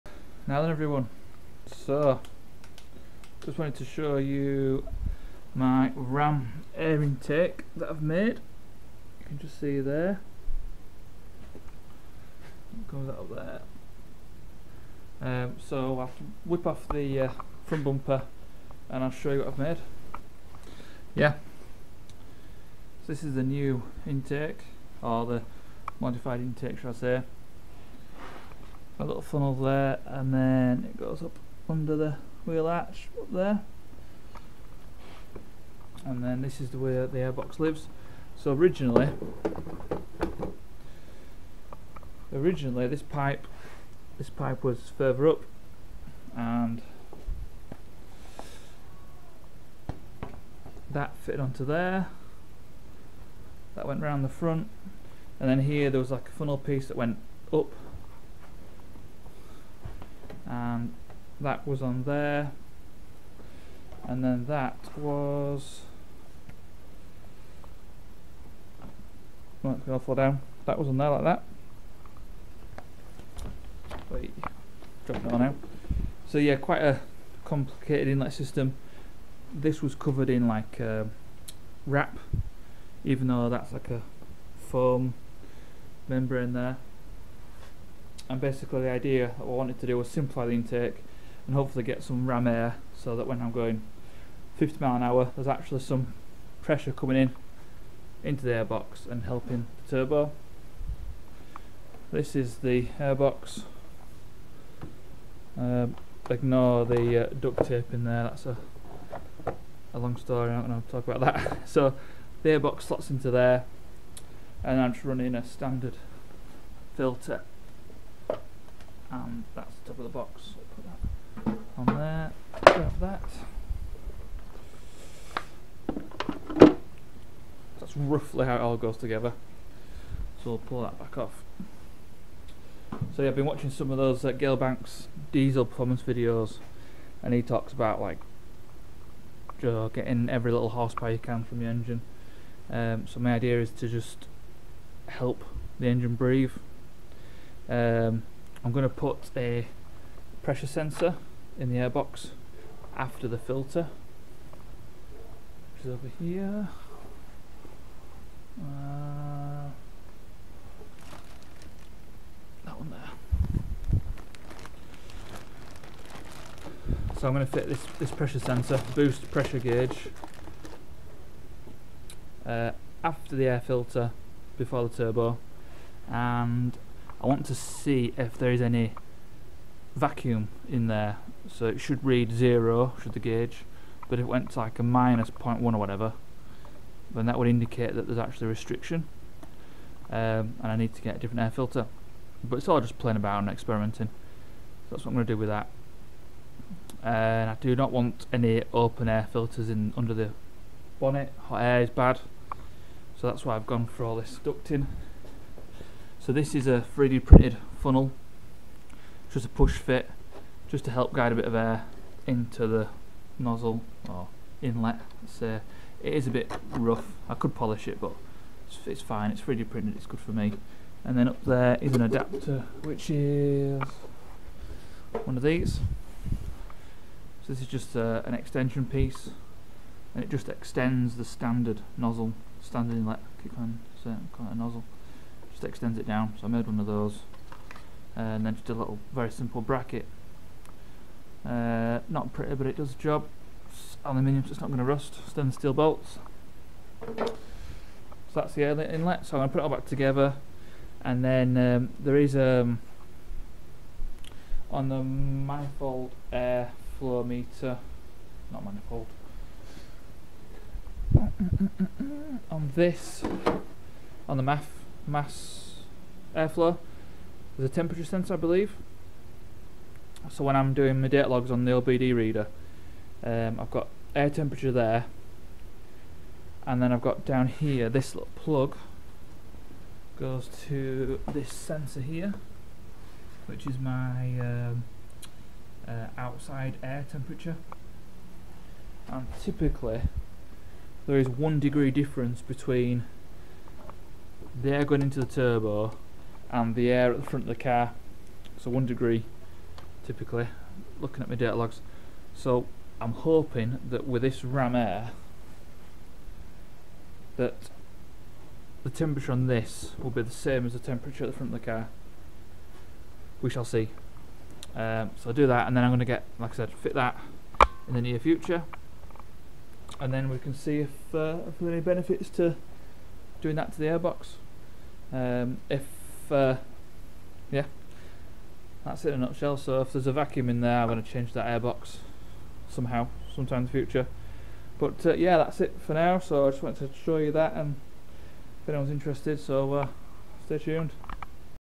3D Printed Ram Air Intake
Just wanted to show you my first attempt at a 3D printed ram air intake modification for my insignia. Ive taken out the drivers side fog light so I have a direct air feed into the new intake for the air filter. Hopefully this will help the engine breath and free up a few more horsepowerss...